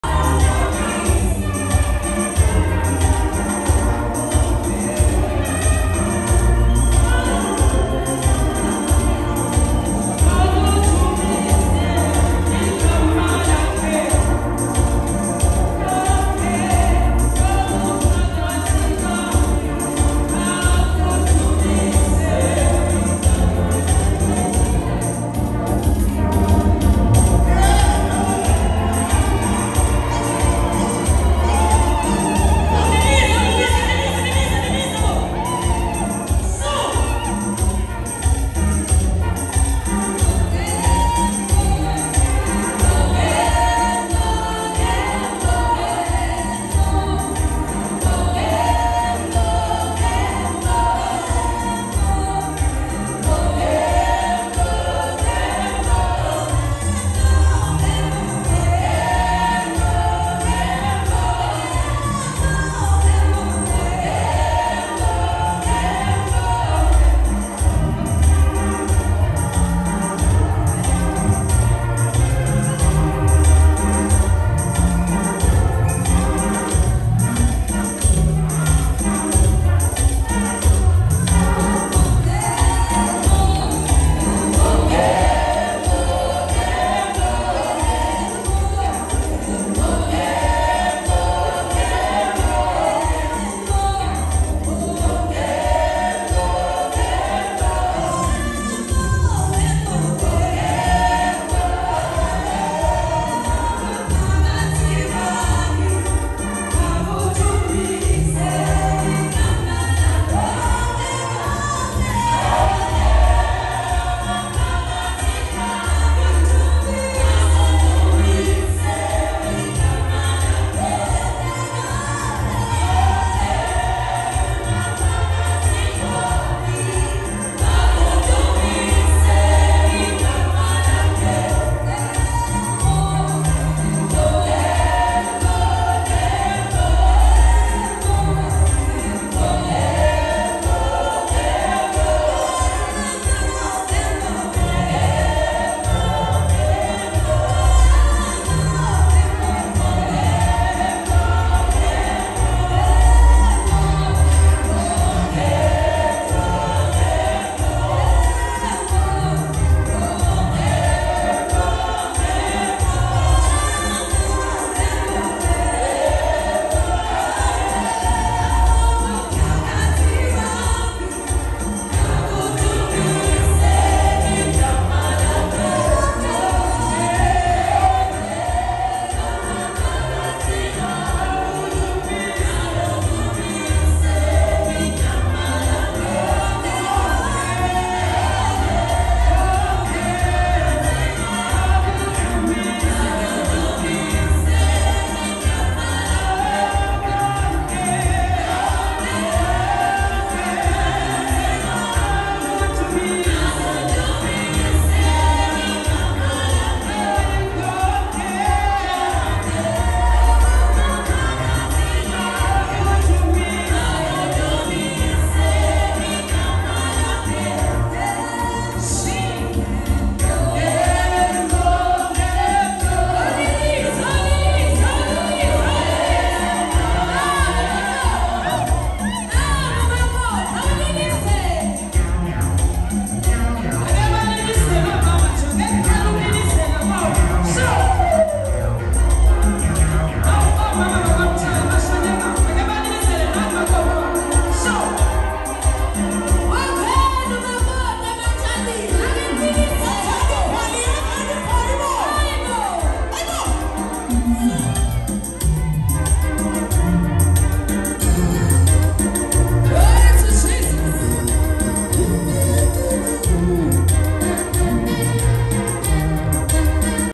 Bride and Groom Reception Grandmarch
The Church of Jesus Christ Wedding Celebration of Fisokuhle Mtshali and Thandi Ngwenya 25 November 2023